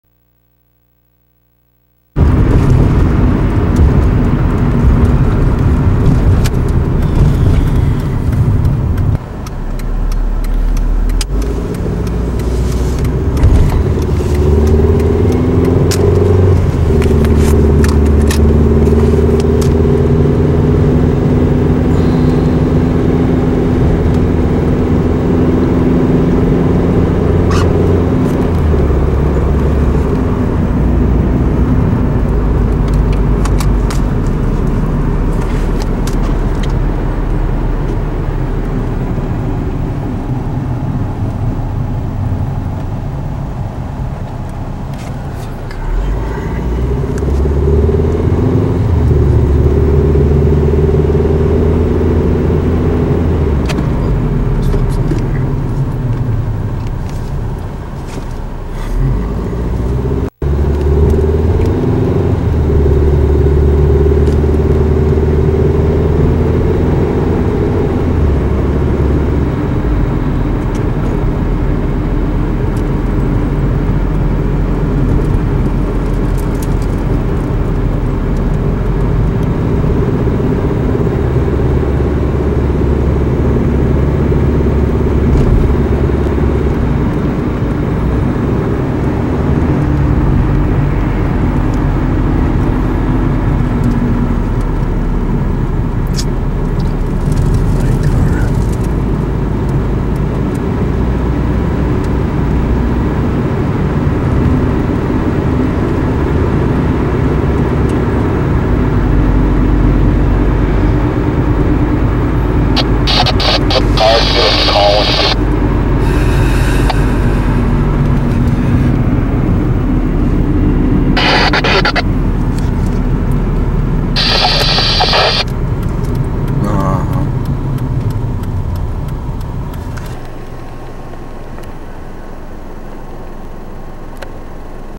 RDMVTL 5970 ~ 1/17/2004 DRIVING WINFIELD ST @ CORNING , NY
CORNING , NEW YORK ~ An eastbound drive along Winfield St from Reynolds Ave to Route 414.
(G)